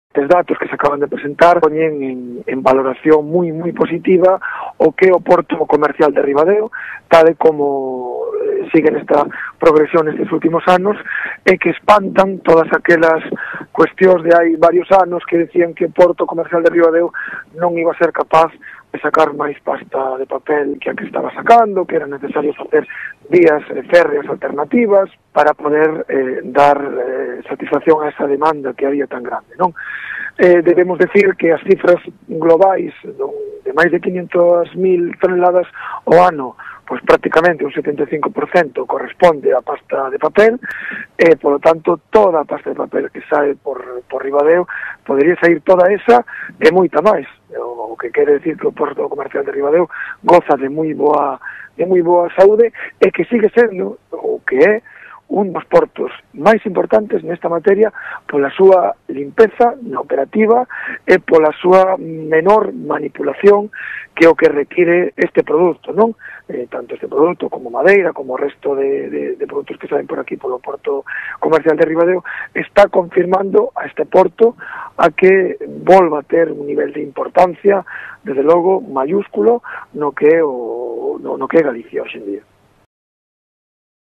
ORT RIBADEO PUERTO COMERCIAL BALANCE OPERACIONES CARGA Y DESCARGA 13-01-2014
El alcalde de Ribadeo, Fernando Suárez, hace una valoración muy positiva de los movimientos registrados en el puerto comercial, después del balance realizado por el presidente de Portos de Galicia sobre las actividades de carga y descarga en los puertos autonómicos. El alcalde ha señalado que Ribadeo el segundo puerto gallego en cuanto a tráfico de mercancías, con 534.126 toneladas, de las que 396.828 fueron de pasta de papel.